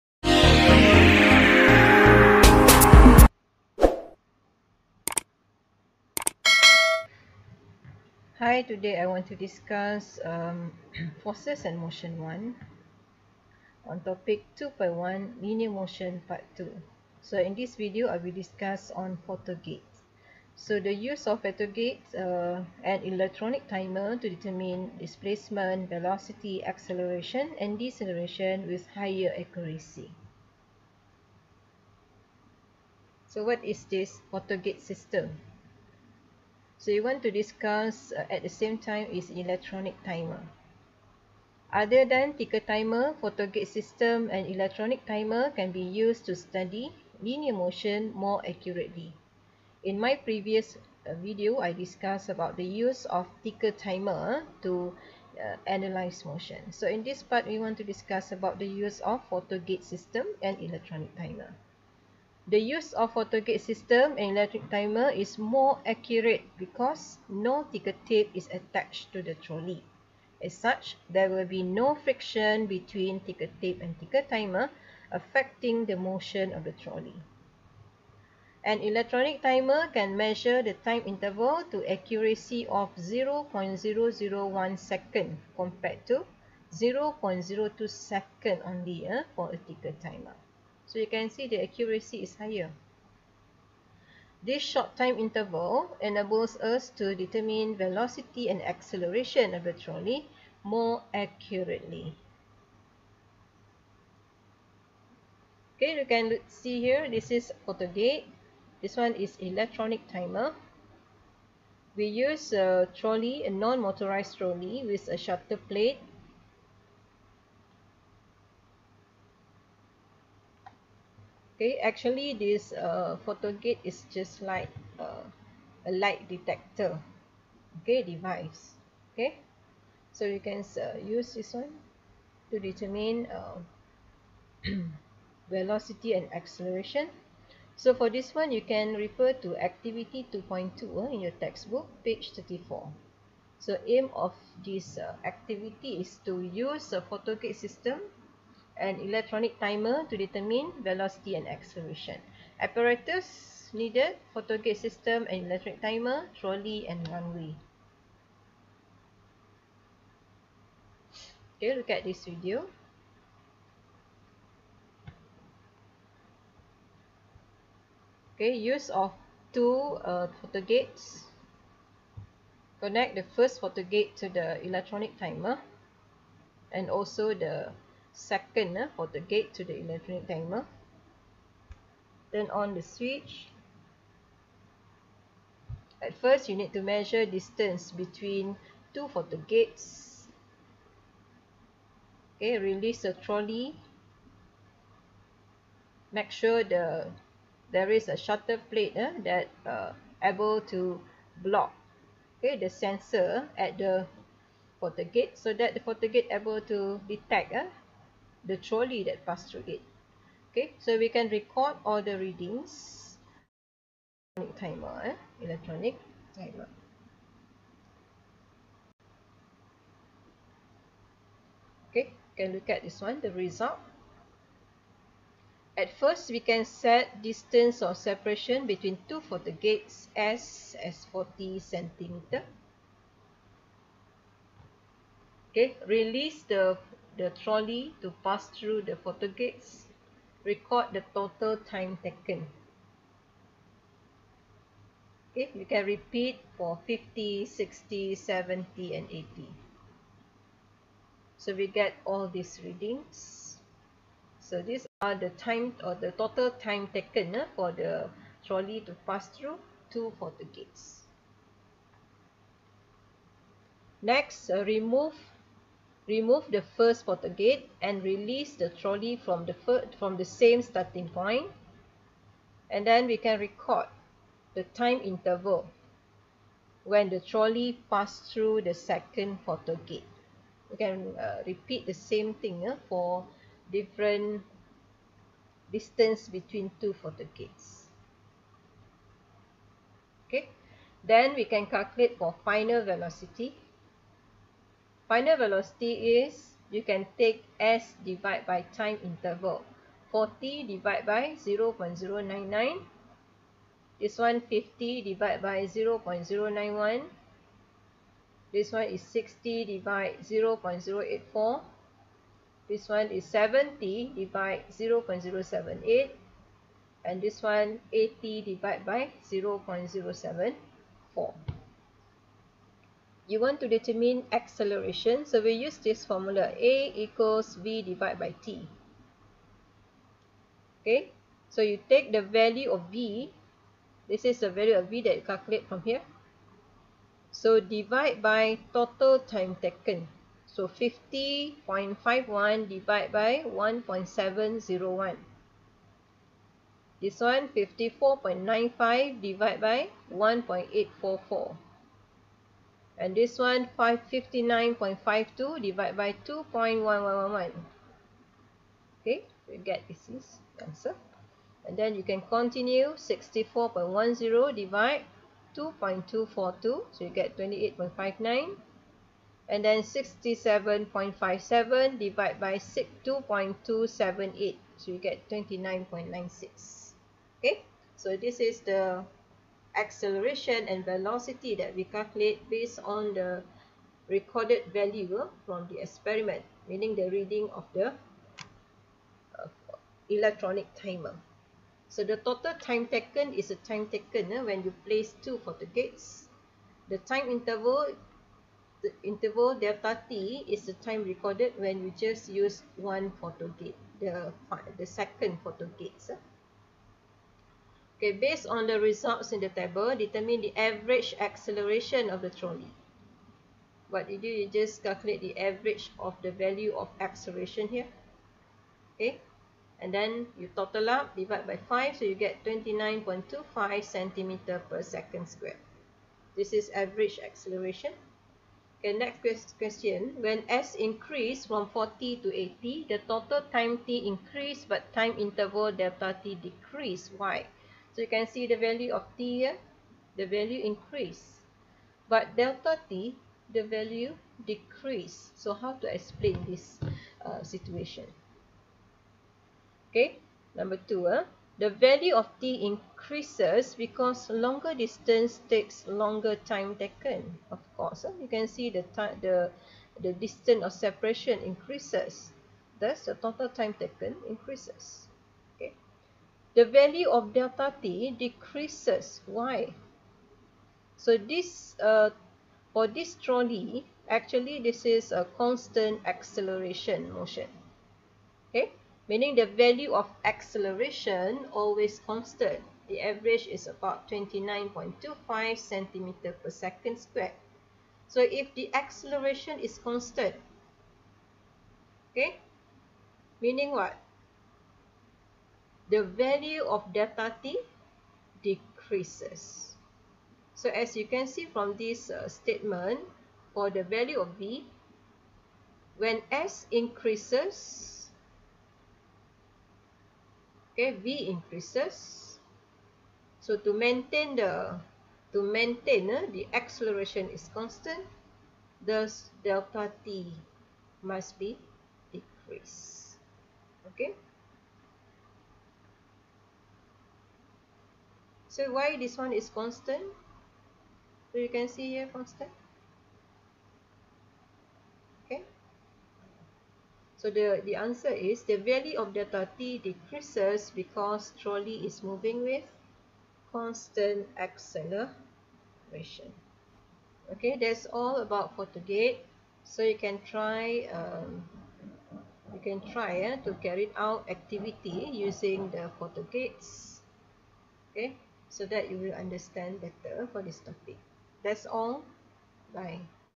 2.1Photogate PhysicsForm4KSSM
Physics Form 4 KSSM
Chapter 2:Forces and motion 1
2.1 Linear motion
This video is my previous video (separated ticker time and photogate)
Hopefully will benefits all students.